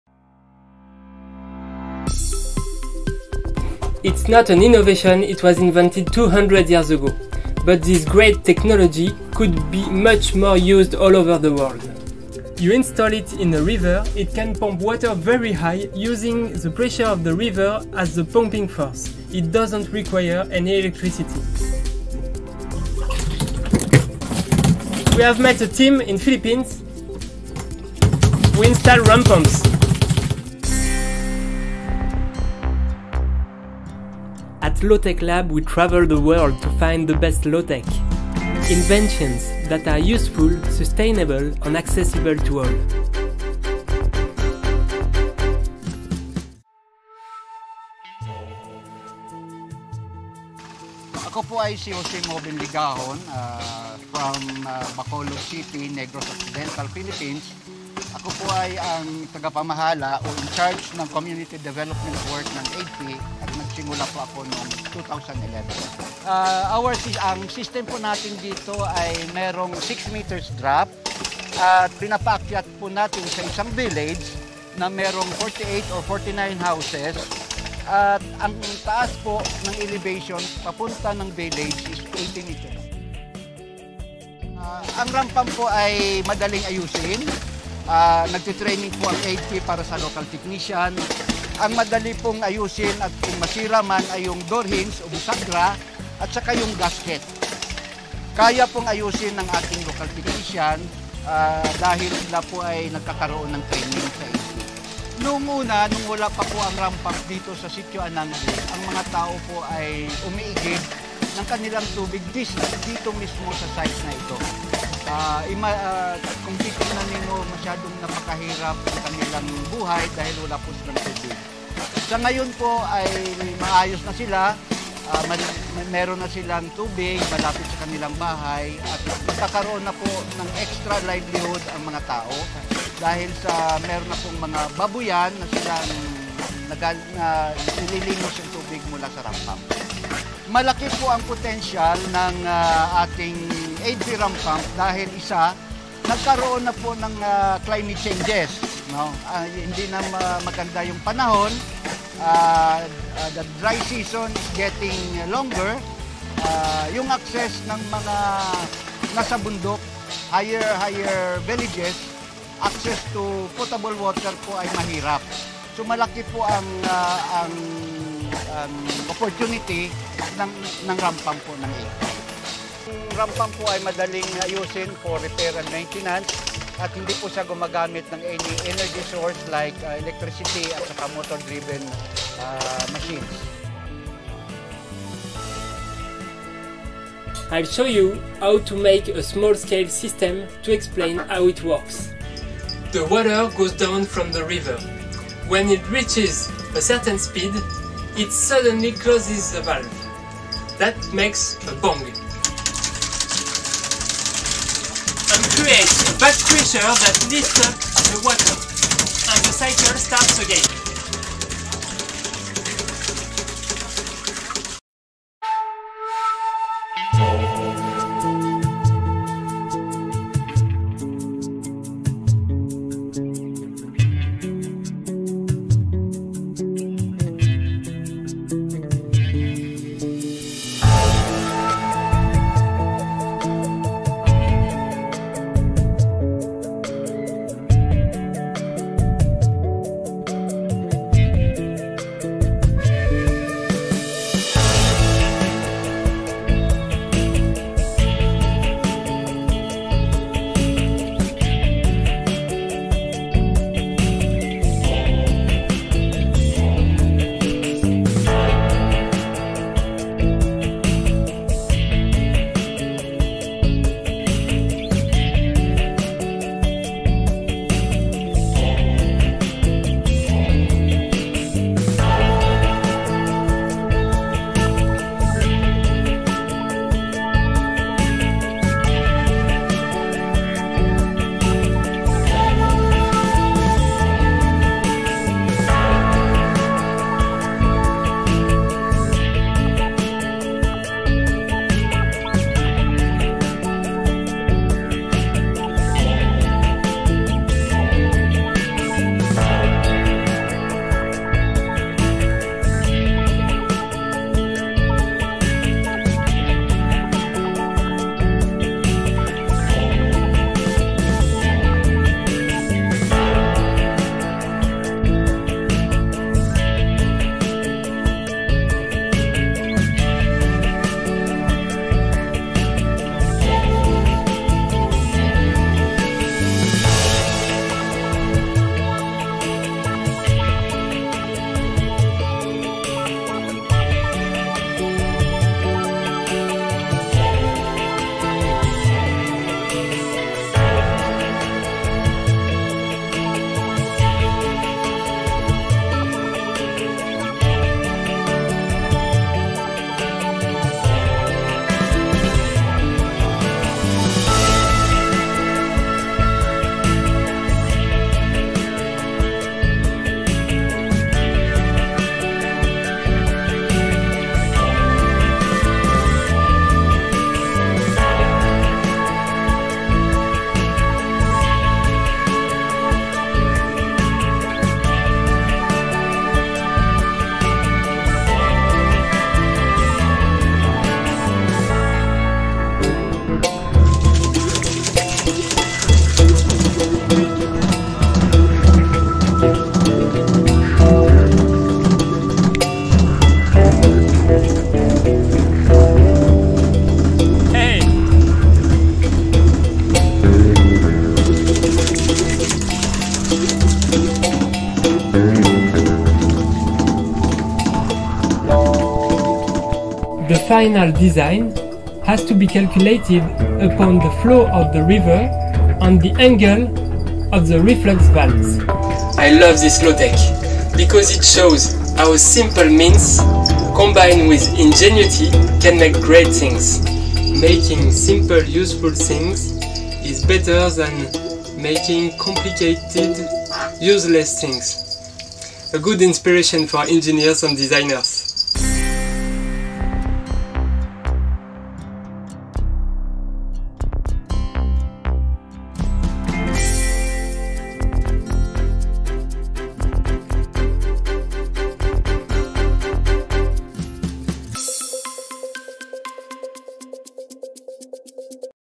How to build a small-scale ram pump - DIY Tutorial
MORE DETAILS ARE GIVEN IN THE SUBTITLES

Today we present you with a system that is more than 200 years old. A pump that works without electricity, without motor: the hydraulic ram-pump 💦
We visited Negros Island in the Philippines where the NGO AIDFI is installing these systems in remote communities without access to safe drinking water 👏

If you think this low-tech deserves to be spread, please help us by adding a thumb up to the video: this will make it more visible for other users. Thank you !